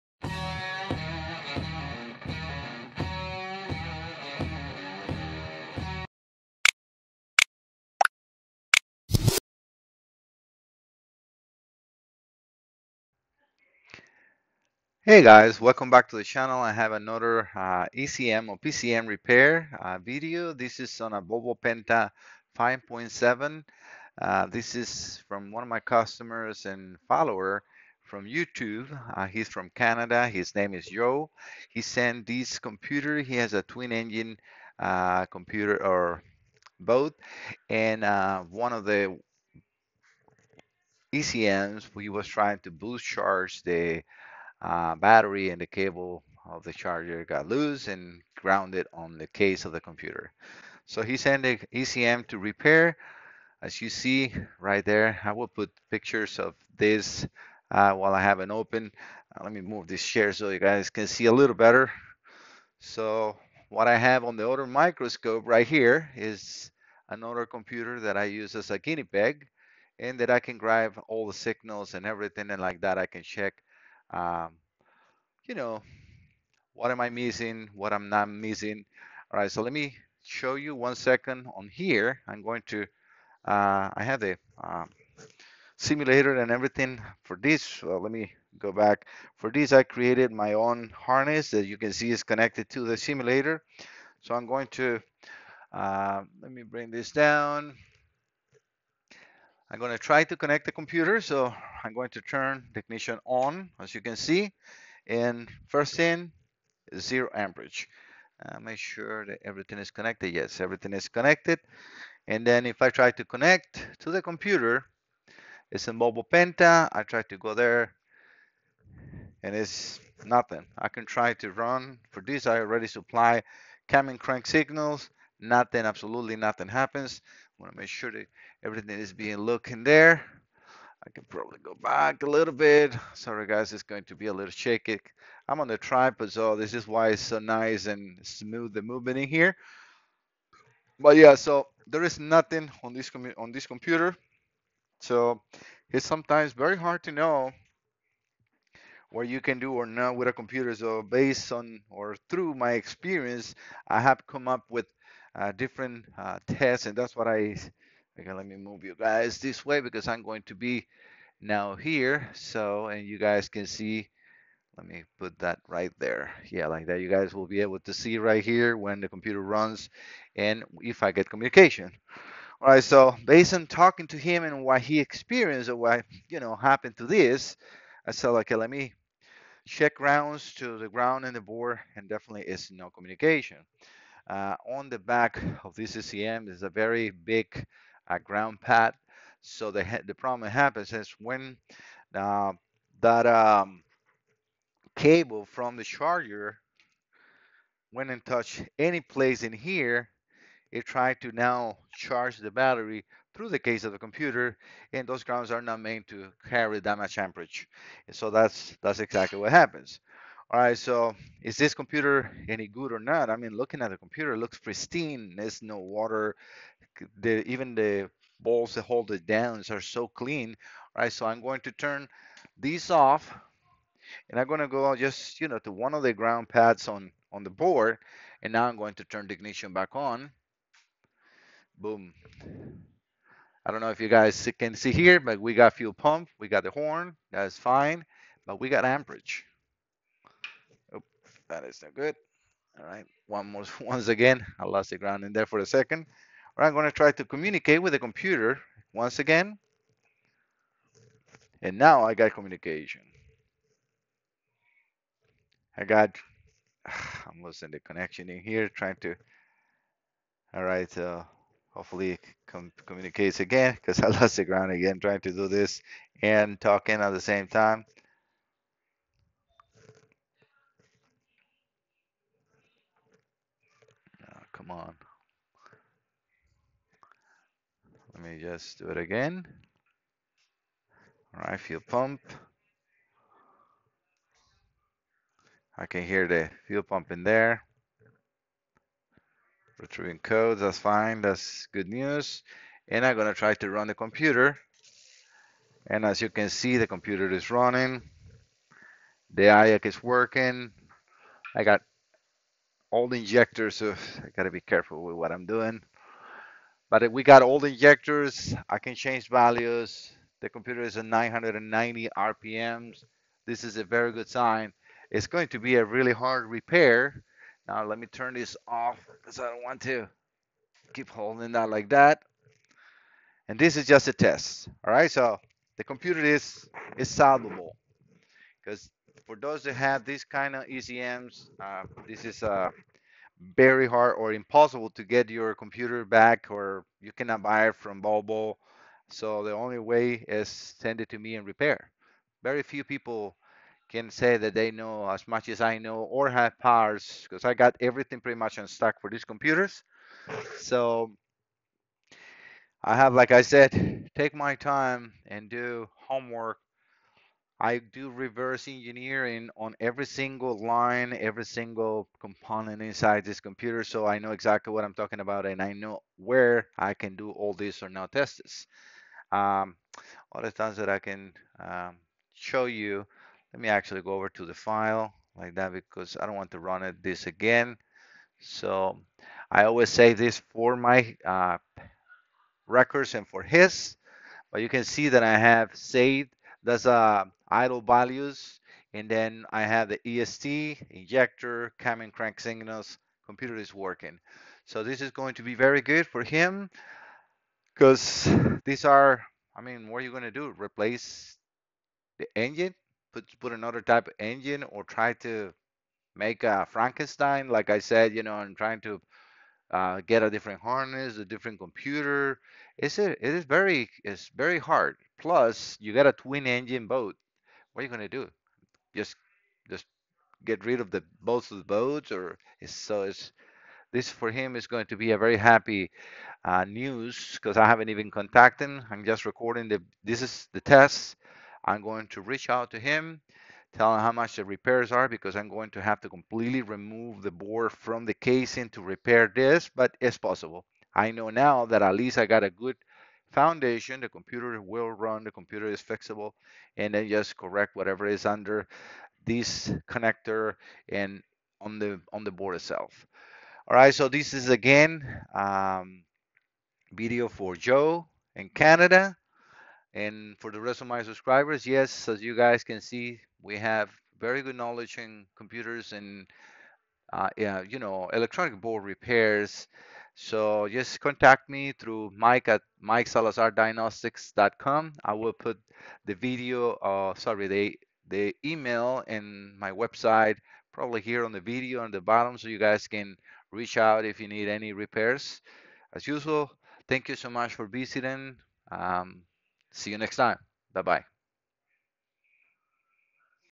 2005 Volvo Penta 5 7L Gi G test ECM no comm Joe Canada
Hi all,
This time a test on a Volvo Penta ECM, this customer is going to be happy. This ECM is no longer available. Enjoy!!